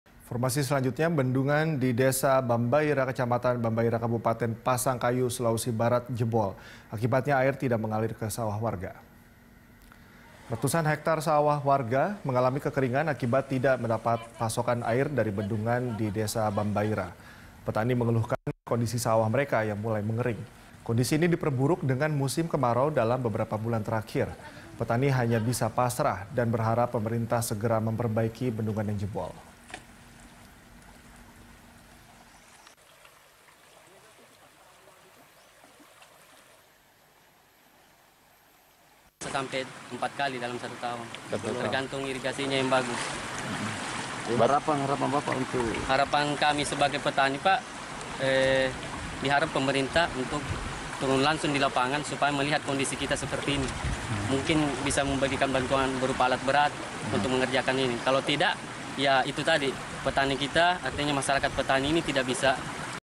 Bendungan di Desa Bambaira Jebol, Hektaran Sawah Warga Kekeringan
Bendungan di Desa Bambaira, Kecamatan Bambaira, Kabupaten Pasangkayu, Sulawesi Barat, jebol. Akibatnya, air tidak mengalir ke sawah warga.

Facebook.com/BeritaSatu
Youtube.com/BeritaSatu